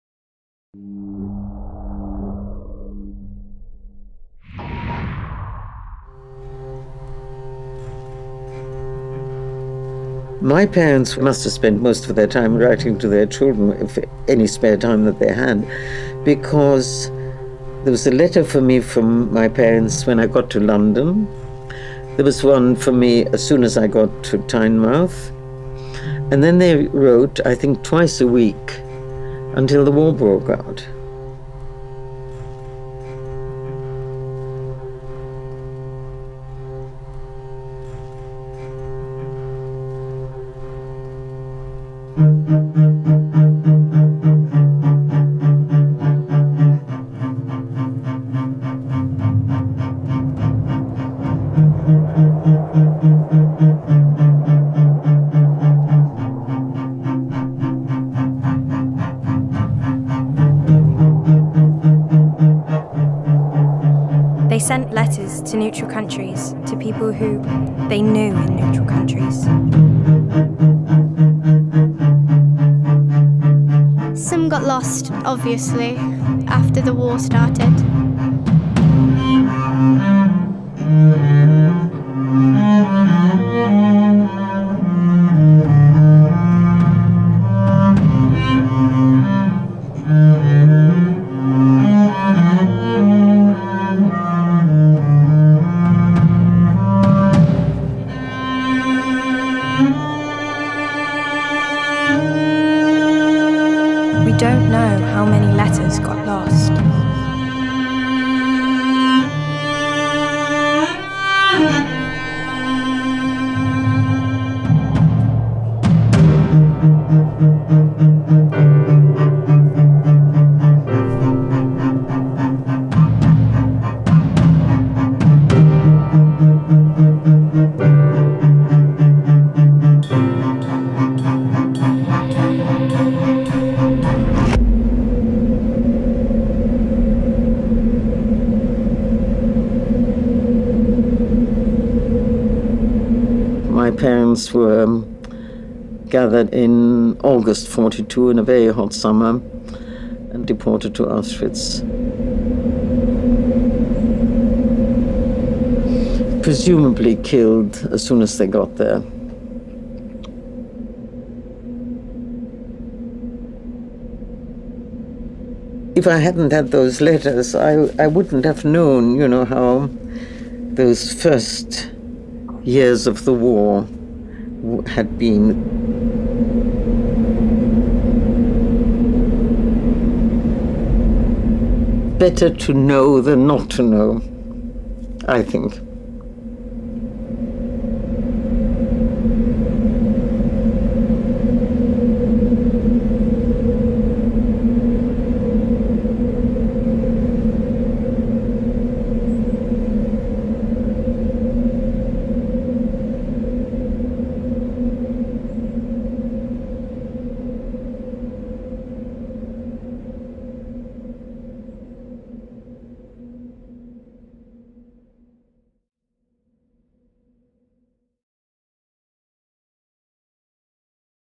Echo Eternal 2019 - The Lifelines
The Lifelines is the result of an artistic residency from Emily Robertson for Highly Sprung Performance Company at Finham Park School, which puts the testimony of Holocaust Survivor Ruth David at the centre of the Echo.

The Echo is the response from the students to the testimony of Holocaust Survivor Ruth David.

Residency artists: Emily Robertson, Highly Sprung
Film: River Rea Films - Mathew Beckett, Jed Codwell and Adam Tate
Survivor - Ruth David

Echo Eternal is a commemorative arts, media and civic engagement project delivered in schools for children of all ages, inspired by the testimony of British survivors of the Holocaust. The testimony was captured by the UK Holocaust Memorial Foundation - UKHMF. The project is underpinned by CORE Education Trust's track record in bringing together diverse communities in a collaborative context, in this case with a common focus of Holocaust memorial and genocide awareness. This focus is supported by the creation of collaborative civic alliances between schools with different social and cultural characteristics.